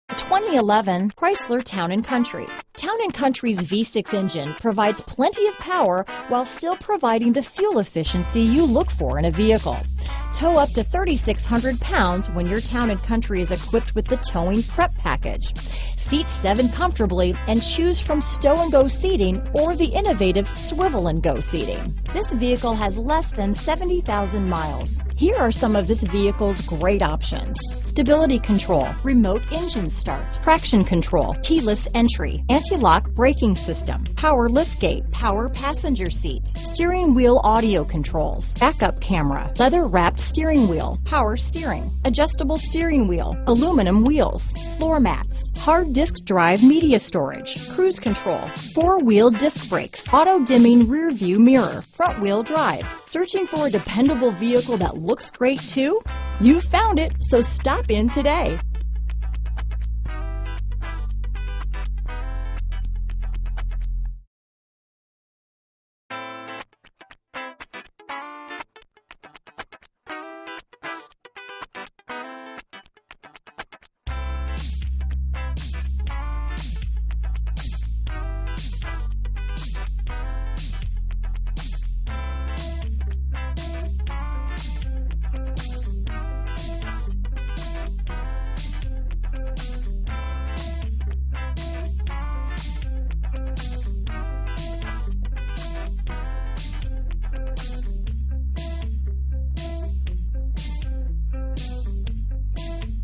2011 Chrysler Town & Country Peoria, Surprise, Avondale, Scottsdale, Phoenix, AZ 45389A
Used 2011 Chrysler Town & Country
2011 Chrysler Town & Country Touring-L - Stock#: 45389A - VIN#: 2A4RR8DG5BR784102

Elegantly expressive, this 2011 Chrysler Town & Country is a meticulous collaboration between pleasantness and polish. With a Gas/Ethanol V6 3.6L/220 engine powering this Automatic transmission, you'll marvel at this unique synergy between the forces of mother nature and the laws of physics. It has the following options: Visors w/illuminated vanity mirrors, Vehicle info center, Variable intermittent windshield wipers, Universal garage door opener, Twist beam axle rear suspension, Tire pressure monitoring display -inc: CORA tire pressure monitoring sensor, Tire carrier winch, Tip start, Tilt/telescoping steering column, and Supplemental front seat side air bags . Visit Moore Chrysler Jeep at 8600 West Bell Road, Peoria, AZ 85382 today.